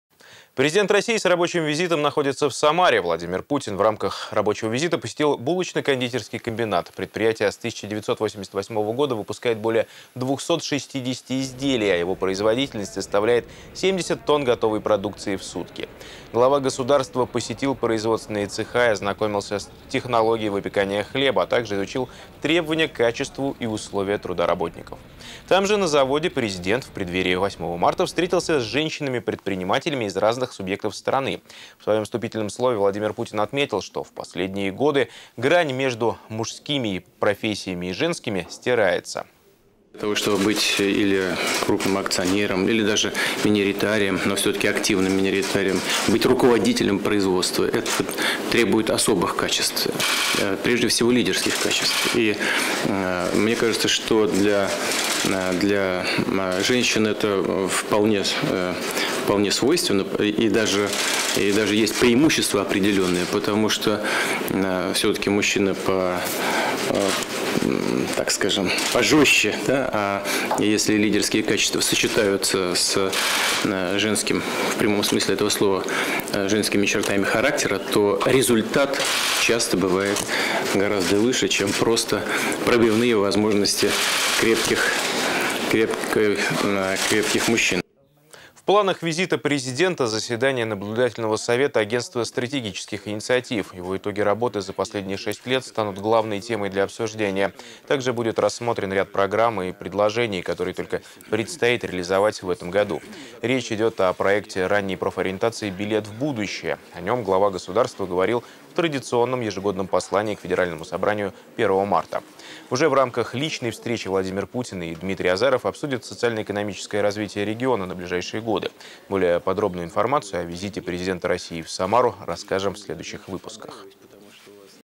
Путин: “Если лидерские качества сочетаются с женскими чертами характера - результат бывает выше"
Президент России с рабочим визитом находится в Самаре. Владимир Путин в рамках рабочего визита посетил булочно-кондитерский комбинат. Предприятие с 1988 года выпускает более 260 изделий, а его производительность составляет 70 тонн готовой продукции в сутки. Глава государства посетил производственные цеха и ознакомился с технологией выпекания хлеба, а также изучил требования к качеству и условия труда работников. Там же, на заводе, президент - в преддверии 8 Марта - встретился с женщинами-предпринимателями из разных субъектов страны. В своем вступительном слове Владимир Путин отметил, что в последние годы грань между мужскими профессиями и женскими стирается.

“Для того чтобы быть или крупным акционером, или руководителем производства, это требует особых качеств, прежде всего лидерских качеств, и мне кажется, что для женщин это вполне свойственно и даже есть преимущество определённое, потому что всё-таки мужчина, так скажем, пожёстче. А если лидерские качества сочетаются с женским, в прямом смысле этого слова, женскими чертами характера, то результат часто бывает гораздо выше, чем просто пробивные возможности крепких мужчин”, - отметил Владимир Путин.

В планах визита президента - заседание наблюдательного совета Агентства стратегических инициатив. Его итоги работы за последние 6 лет станут главной темой для обсуждения. Также будет рассмотрен ряд программ и предложений, которые только предстоит реализовать в этом году. Речь идет о проекте ранней профориентации "Билет в будущее". О нём глава государства говорил в традиционном ежегодном Послании к Федеральному Собранию 1 марта. Уже в рамках личной встречи Владимир Путин и Дмитрий Азаров обсудят социально-экономическое развитие региона на ближайшие годы. Более подробную информацию о визите президента России в Самару, расскажем в следующих выпусках.